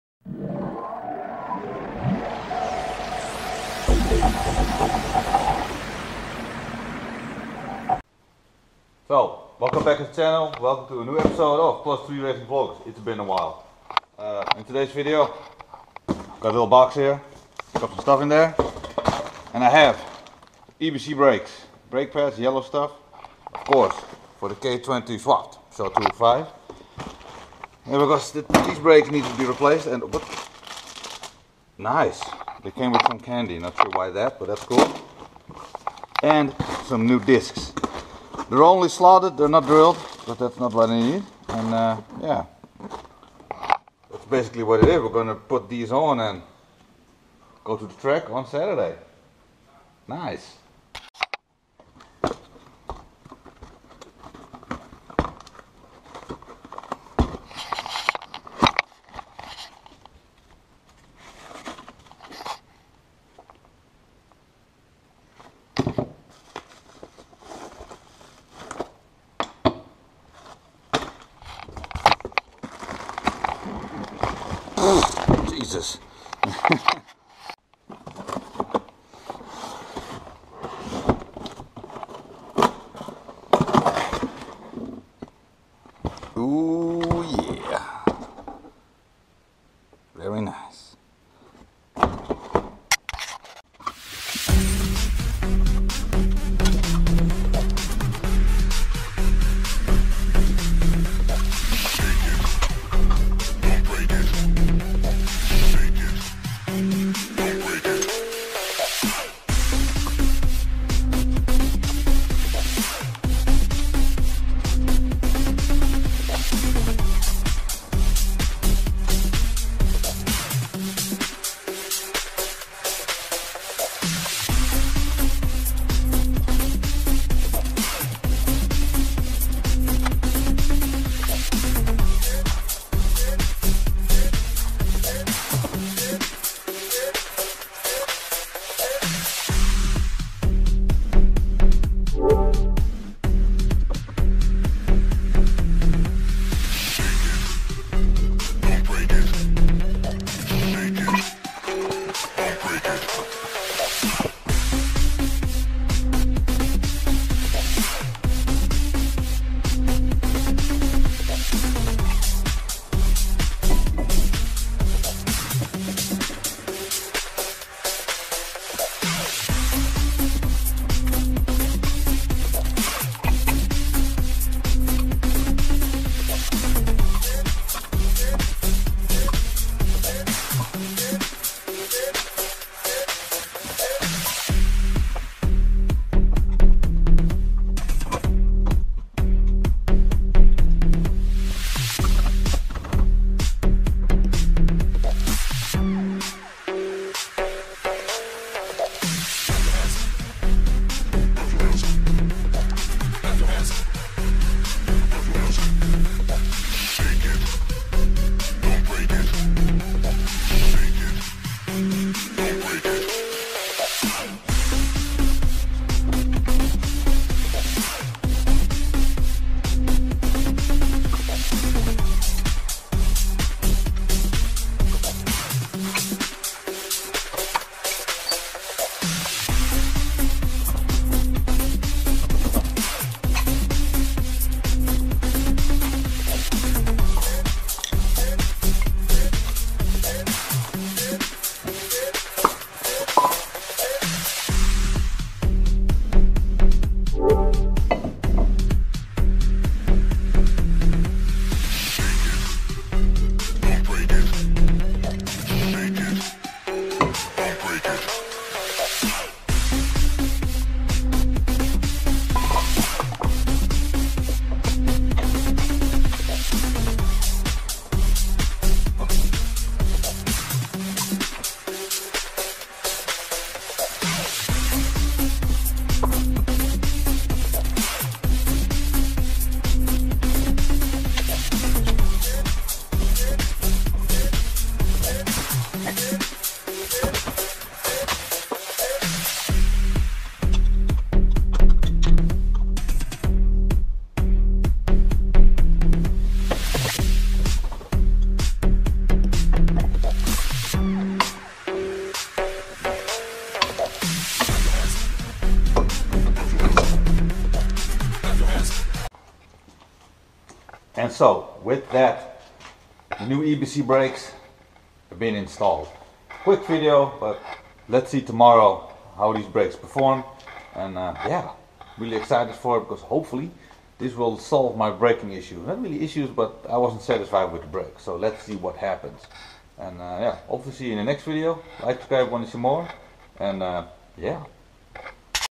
EBC brake upgrade for the K20 205! // +3 Racing Vlogs
yah this a short video of the new brakes, hopefully they are an massive improvment